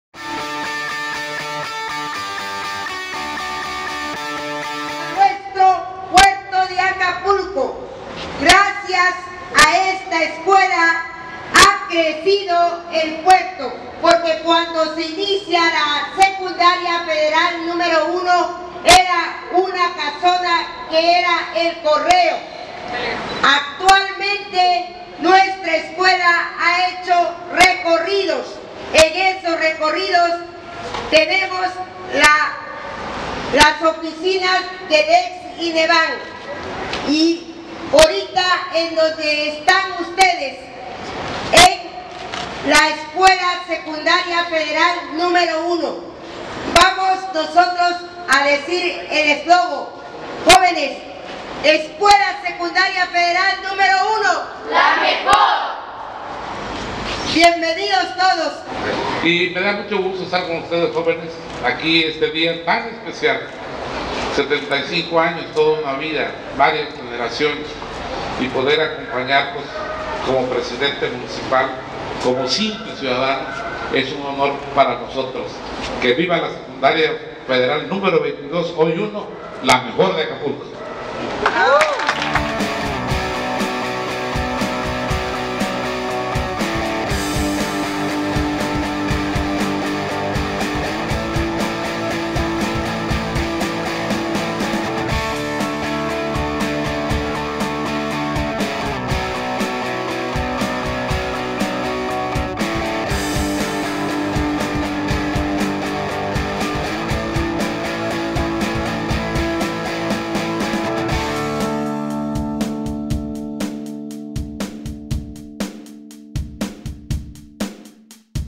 75° Aniversario Escuela Secundaria Federal No 1
Inician los festejos del 75° Aniversario de la Escuela Secundaria Federal No 1 con una caminata deportiva y como invitado de honor acudió el presidente municipal Luis Walton, quien es alumno egresado de la generación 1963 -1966. Por tal motivo se le hizo entrega del cetro para posteriormente trasladarse al Malecón del puerto, pasar el cetro a la directora del plantel y dar inicio a la caminata.